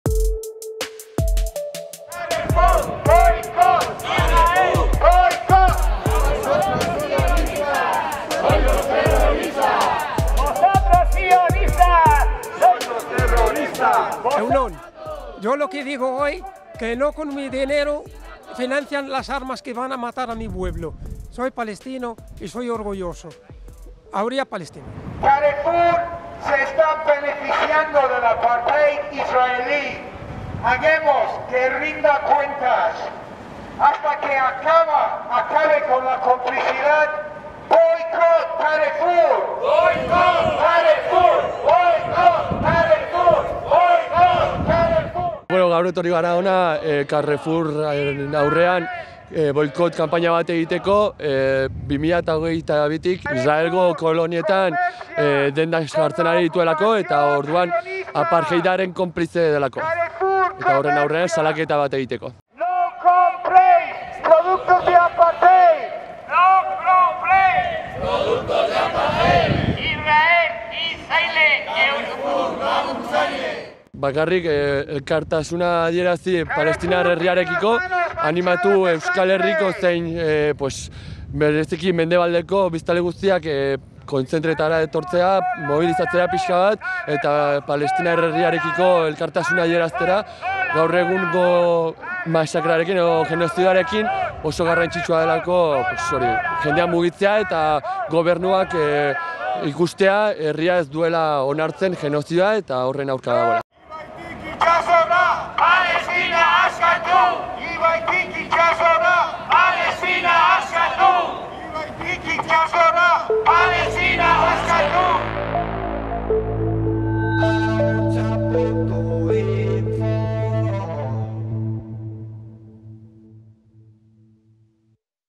Carrefour kateari boikot egitea eskatu dute, Israelekin duen loturagatik
Boikot, Desinbertsioak eta Zigorrak taldeko kideak Israel Palestinan egiten ari den genozidioa salatzeko Iruñeko Mendebaldeako auzoan dagoen Carrefour merkatalgunean elkartu dira. Kate honek Israelgo Electra Consumer eta Yenot Bitan enpresekin akordioak ditu, eta kate horiek lurralde okupatuetan kokatzen diren kolonietan presentzia dute.